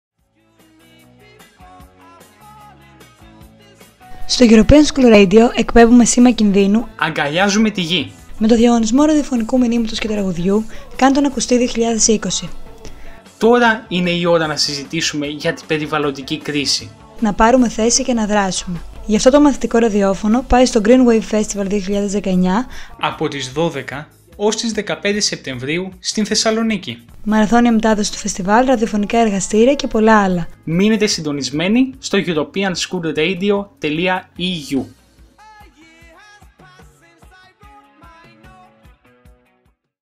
ESRadio @ GreenWave Festival 2019
Διακόπτουμε την ροή του προγράμματος μας ! Εκπέμπουμε SOS! Ο πλανήτης αργοπεθαίνει και ο χρόνος που έχουμε για να τον σώσουμε τελειώνει!

H νέα ραδιοφωνική χρονιά ξεκινά με την ανακοίνωση του διαγωνισμού "Καν΄το ν'ακουστεί 2020" στο Greenwave festival 2019 στη Θεσσαλονίκη !